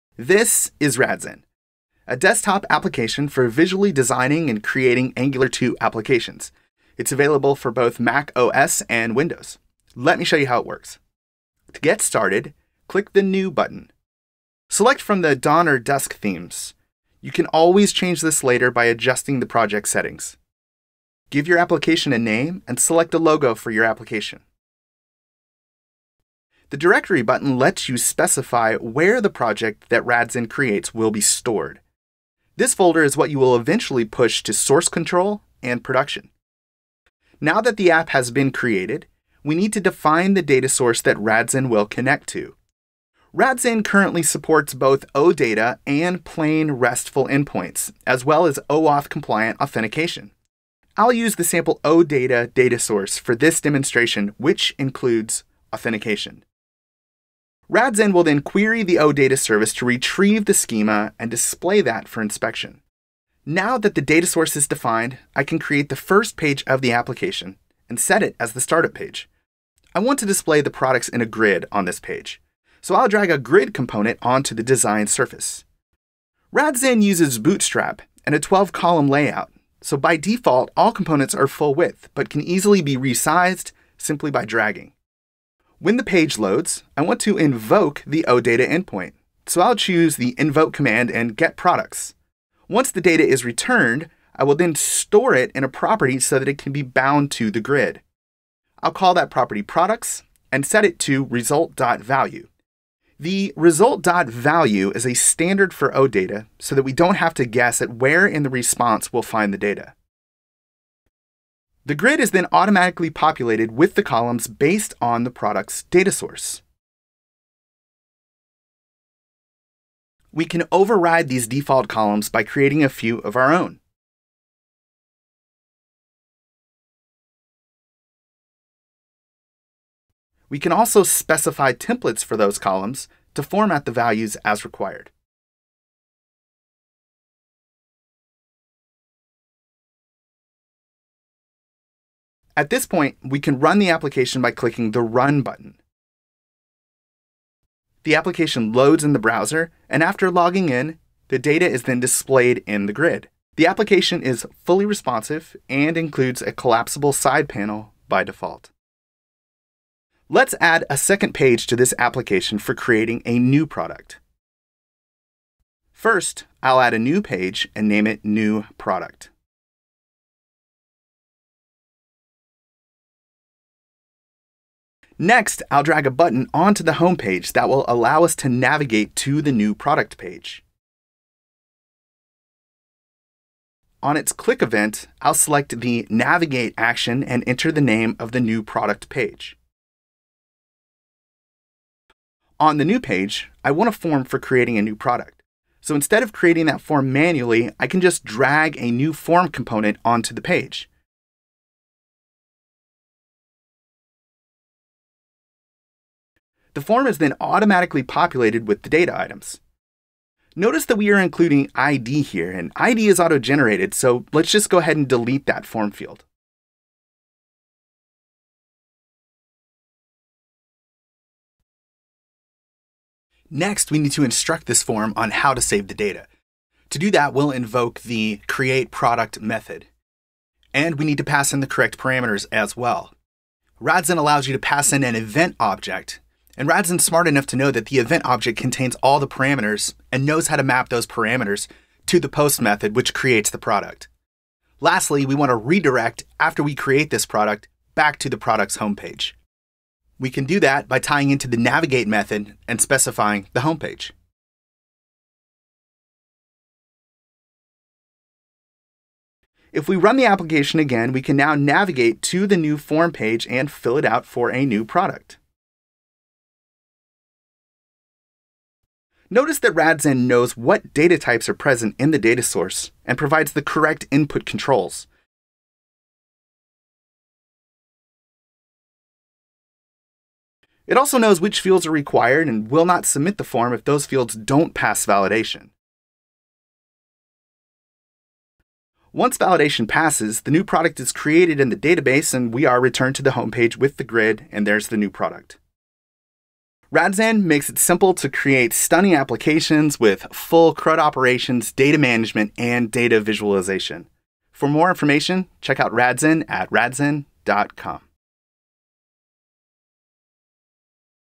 Radzen - The first rapid application development tool for Angular 2
Radzen is the first desktop tool for visually designing and creating Angular 2 applications. . The software is available for both Mac OS and Windows. With just a few clicks you can create your project and define its data source. Radzen currently supports

► OData
► RESTful endpoints
► OAuth compliant authentication

Building your application is easy - just drag and drop the elements you need from the left sidebar to the design surface. You can choose from grids, pie charts, forms, bar charts and more. Radzen uses bootstrap and a 12-column layout so by default all components are full-width but can easily be resized simply by dragging.

Use the right panel to input commands and automatically populate your elements with data. You can run the application in the browser. It is fully responsive and includes a collapsible side panel by default.

Radzen makes it simple to create stunning web applications with full CRUD operations, data management and data visualization.